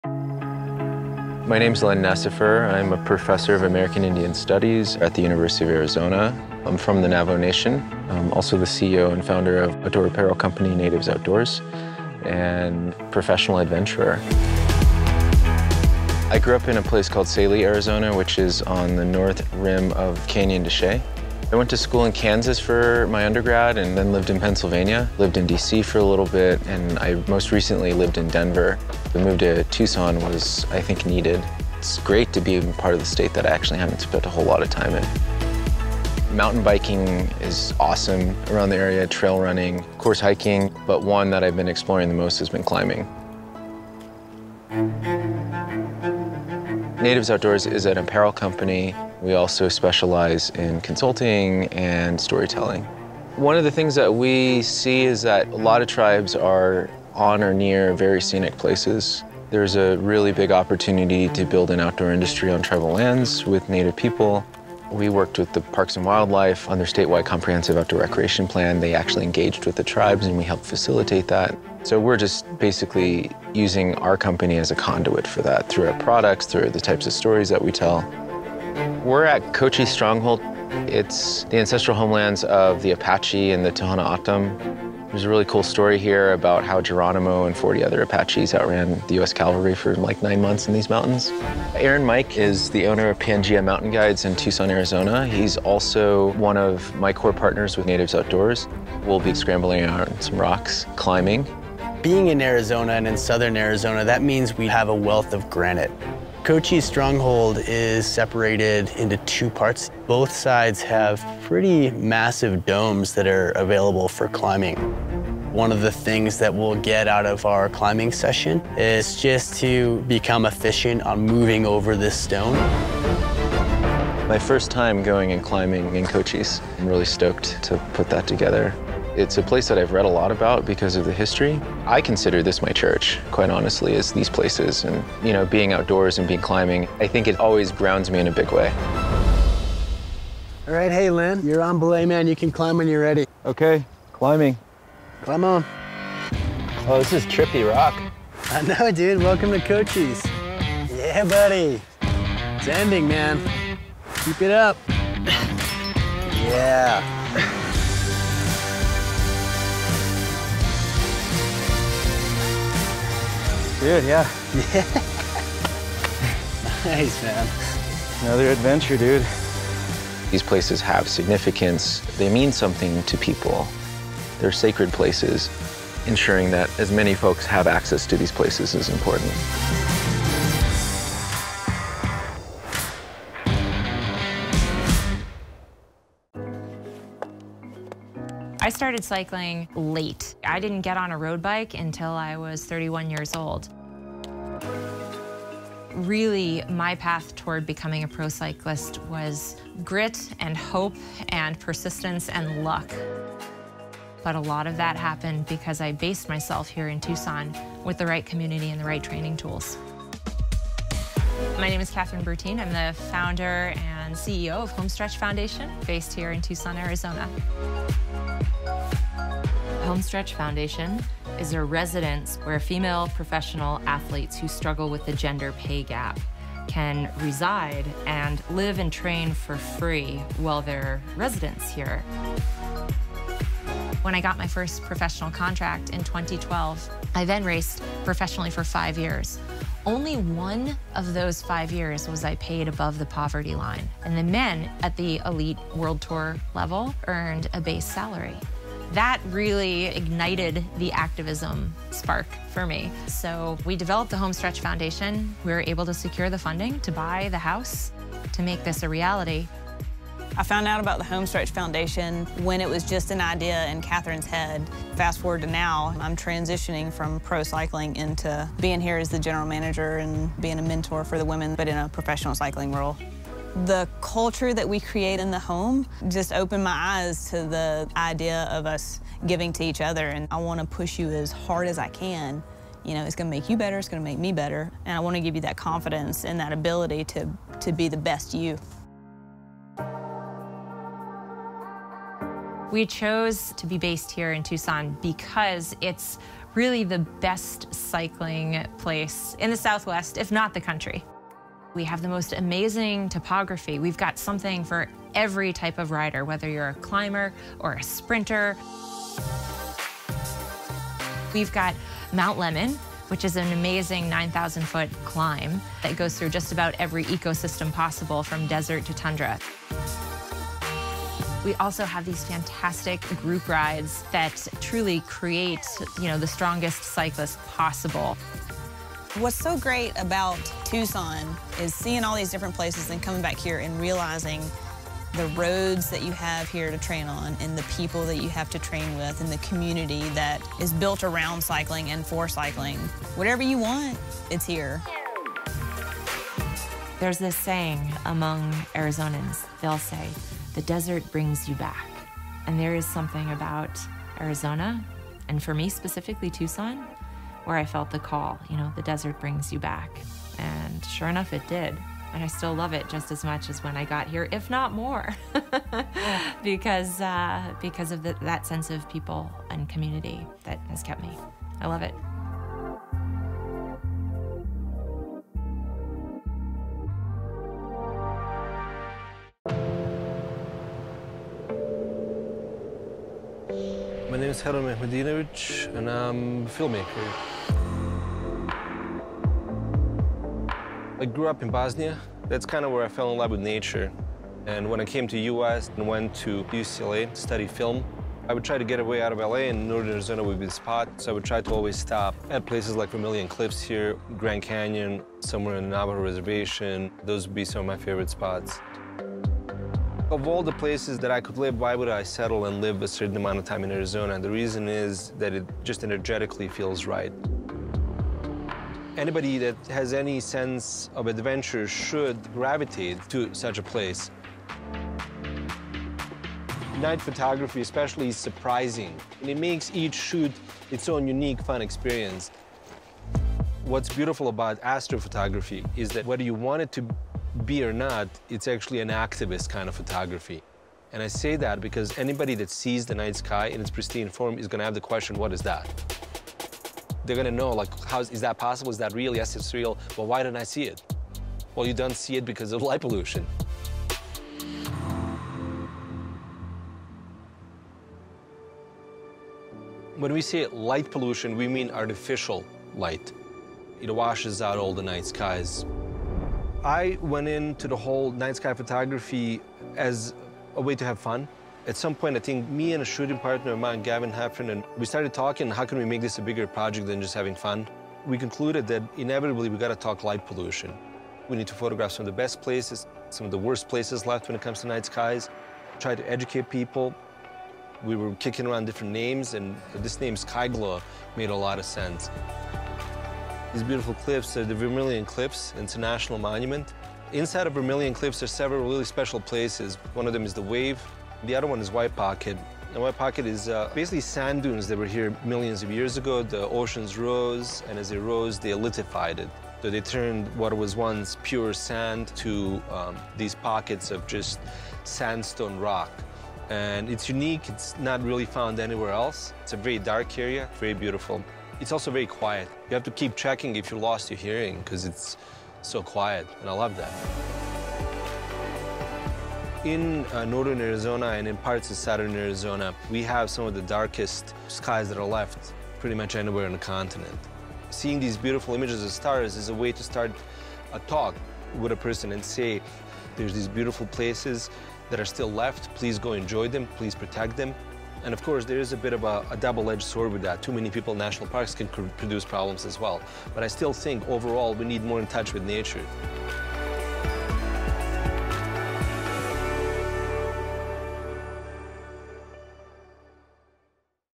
Arizona Disrupters
Three Arizona disrupters are working to change the status quo in the use of tribal lands, pay parity in professional women's cycling, and preservation of the night skies.